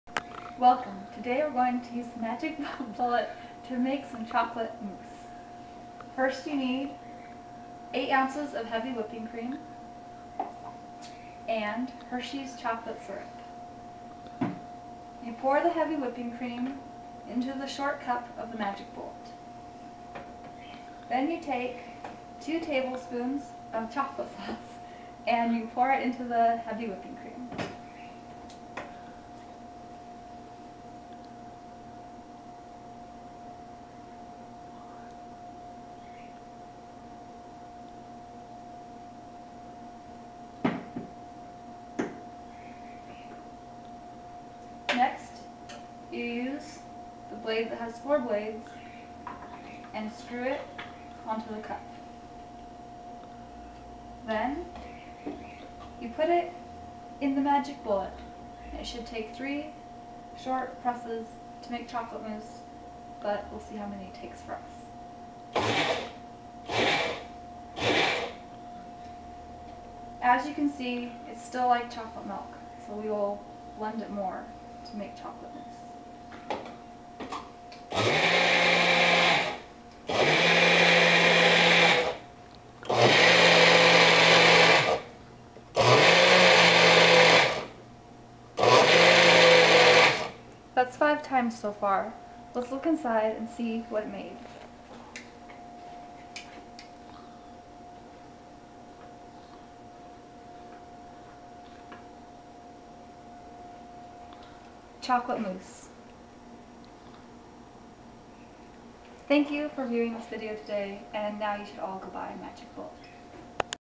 Magic Bullet magic
Testing out the new Magic Bullet I got for $40 at Costco over the wknd. Making - Chocolate Mousse!